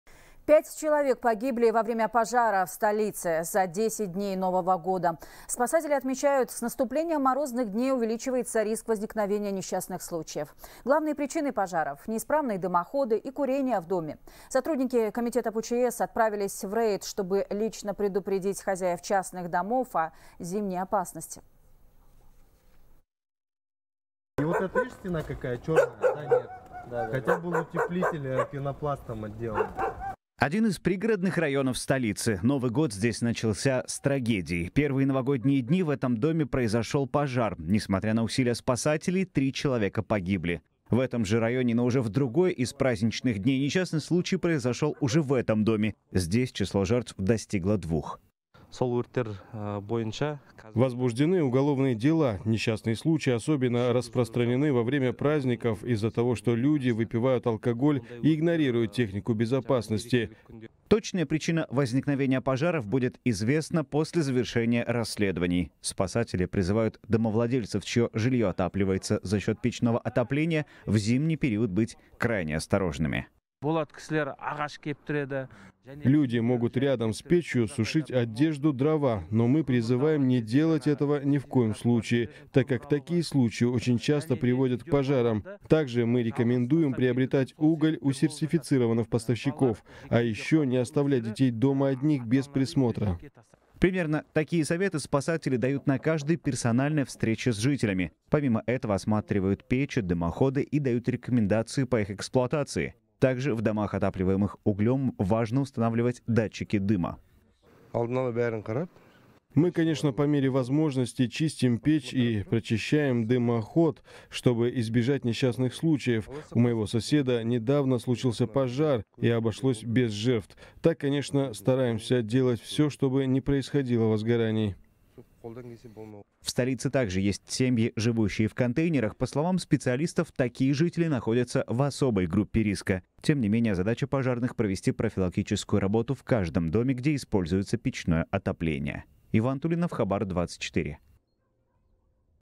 5 человек погибли в пожарах с начала года в Нур-Султане
Пять человек погибли во время пожаров в столице за 10 дней нового года, передает корреспондент «Хабар 24». Спасатели отмечают, что с наступлением морозных дней увеличивается риск возникновения несчастных случаев. Главные причины пожаров – неисправные дымоходы и курение в доме. Сотрудники Комитета по ЧС отправились в рейд, чтобы лично предупредить хозяев частных домов о зимней опасности.